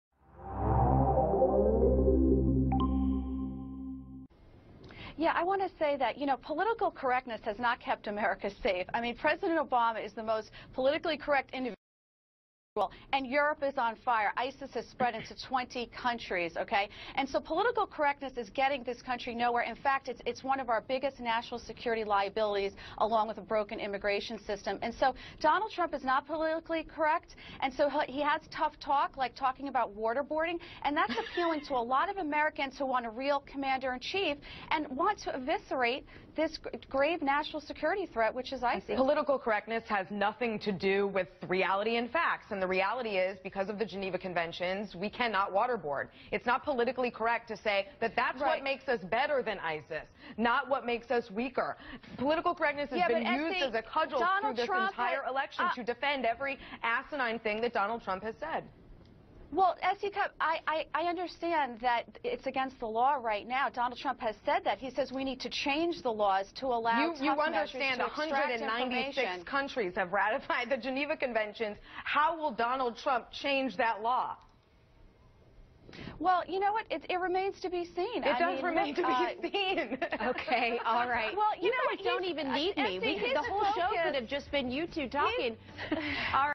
Cupp to Trump Supporter: Stop Using ‘Political Correctness’ as Cudgel to Defend Trump!
CNN’s S.E. Cupp clashed with a Donald Trump supporter this afternoon who kept defending the GOP frontrunner by repeatedly playing the political correctness card.

Adriana Cohen kept going on about how “political correctness has not kept America safe” and “political correctness is getting this country nowhere” and “Donald Trump is not politically correct, and so he has tough talk, like talking about waterboarding.”

Cupp shot back by saing, “Political correctness has nothing to do with reality and facts… Political correctness has been used as a cudgel through this entire election to defend every asinine thing DOnald Trump has said.”

She pointed out that torture is illegal. Cohen said the laws nee to be changed, but Cupp pointed out almost 200 nations have signed onto international torture laws and asked how Trump would change that.
Cohen said, “It remains to be seen.” Cupp laughed.

Watch above, via CNN.